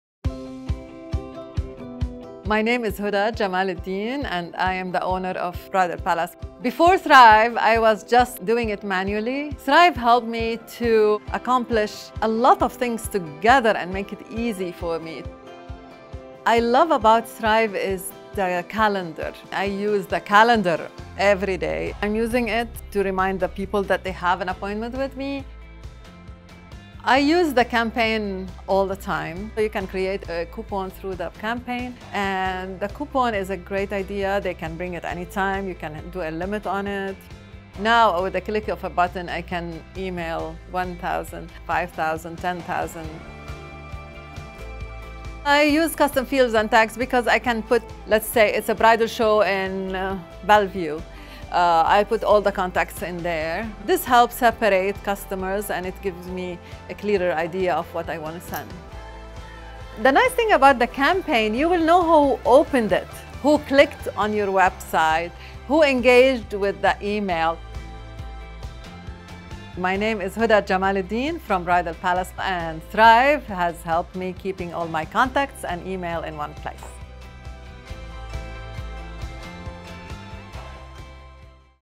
Bellevue Bridal Palace
High Thryv Award Winner Bellevue Bridal Palace educates business owners how Thryv's CRM, Calendar, Campaigns, Email, Tags and Custom Fields and Reporting streamline its business.